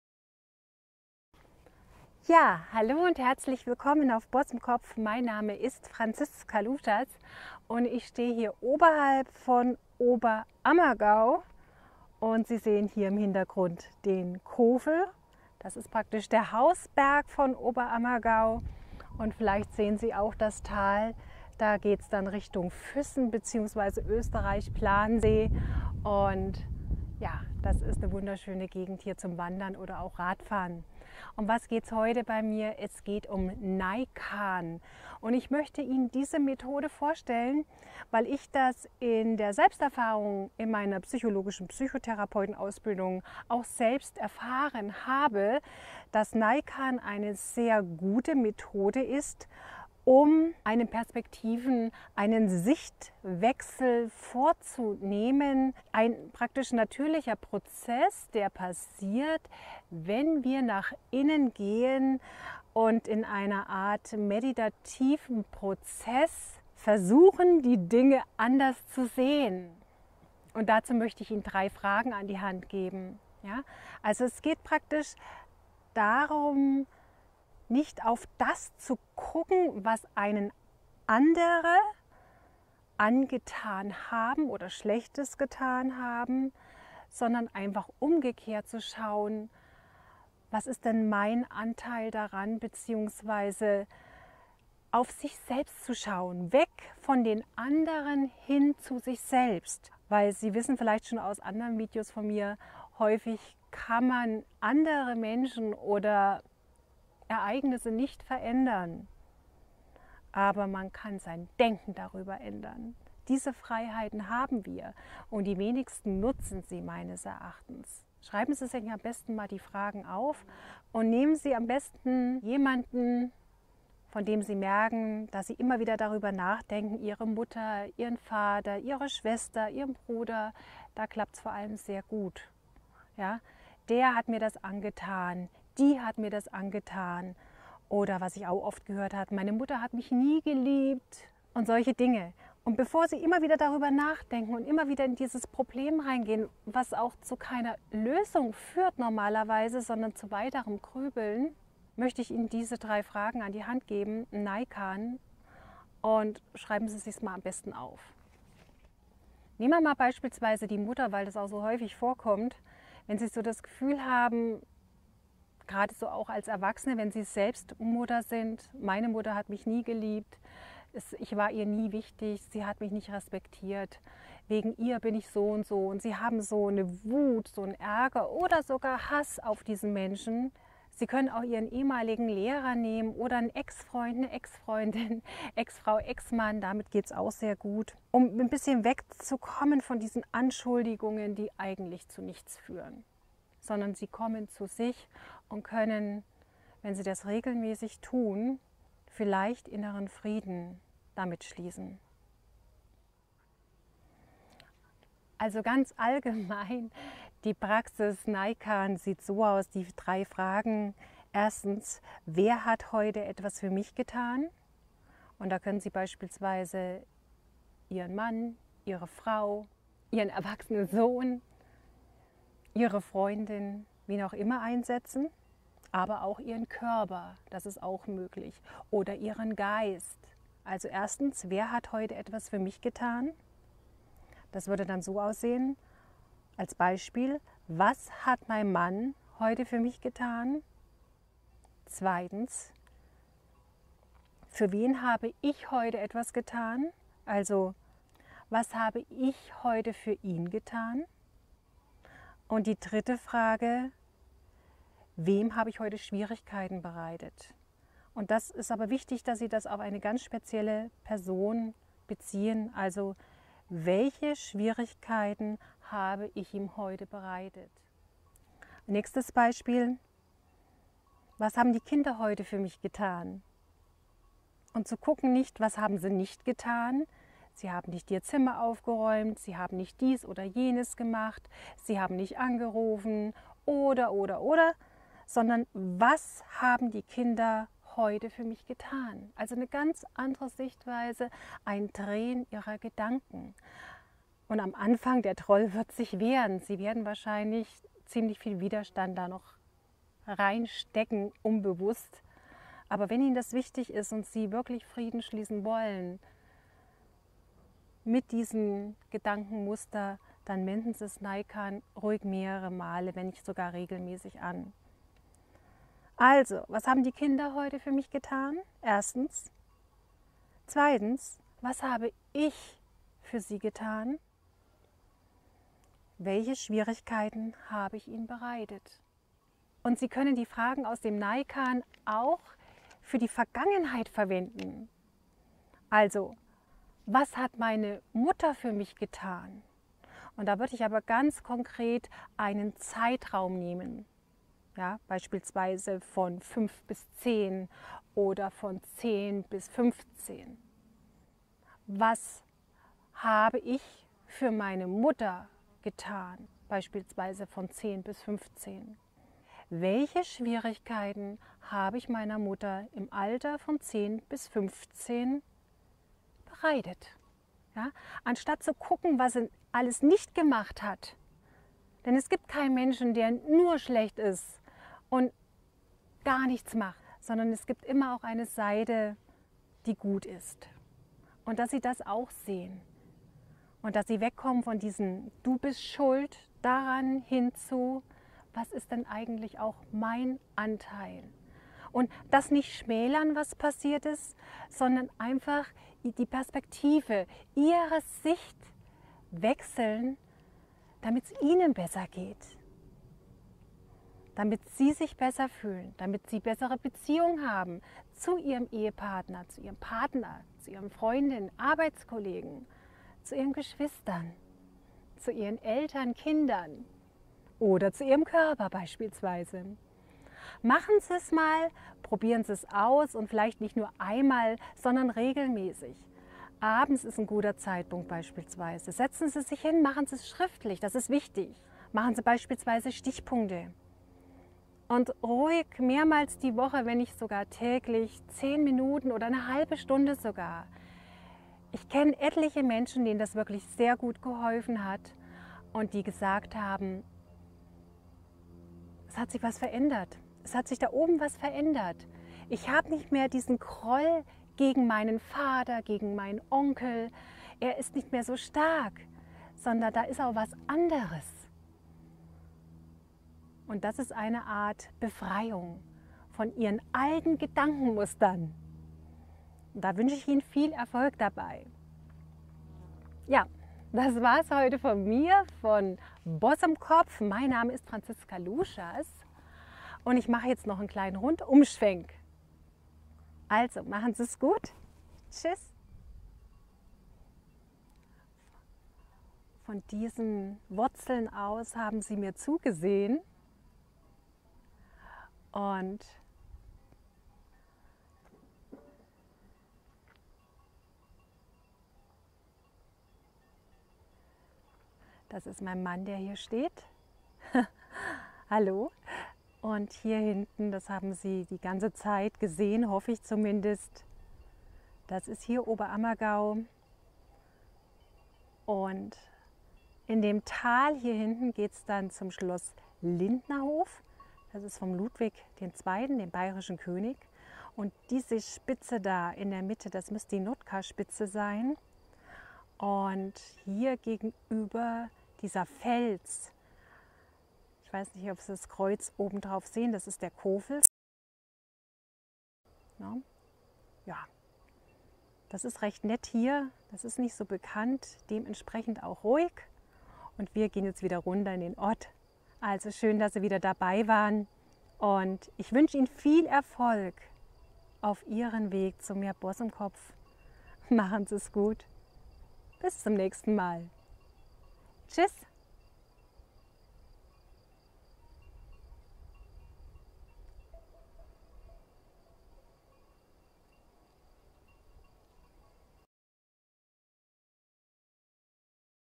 NAIKAN - Alte negative Gefühle aus der Vergangenheit | BossImKopf
Vergangenes loslassen mit NAIKAN

Ich kenne viele Menschen, die auch nach langer Zeit noch einen tiefen Groll gegen einen bestimmten, oft nahestehenden Menschen wie z.B. Mutter, Vater, Bruder oder Schwester haben.

Sie geben dem anderen die Schuld für ihre schlechten Gefühle und ärgern sich - oft auch noch viele Jahre später - über das, was ihnen angetan worden ist.

Sie fühlen sich von diesem Menschen ungeliebt, nicht anerkannt, ausgenutzt, ungerecht behandelt, bevormundet oder im Stich gelassen.

Falls noch Kontakt zu diesem Menschen besteht, dann ist dieser in der Regel geprägt von Vorwürfen, Ärger, Frustration, Traurigkeit, Enttäuschung oder Wut.

Das wiederum wirkt sich nicht nur negativ auf ihre Gefühle, ihre Lebensqualität oder ihren Körper aus. Sondern belastet häufig auch ihre anderen Beziehungen  wie z.B. zu Mann, Frau, Kindern oder Freunde.

Eine gute Methode, sich aus diesem alten Muster zu befreien, ist NAIKAN.

Wie du dich durch NAIKAN mit deiner Vergangenheit (z.B. Mutter, Vater, Geschwister) ein Stück weit versöhnen kannst, erzähle ich dir in diesem Video.

Ich wünsche dir viel Erfolg beim Boss im Kopf werden und bleibe weiter dran, denn der richtige Weg lohnt sich 😊

Franziska Luschas
Diplom-Psychologin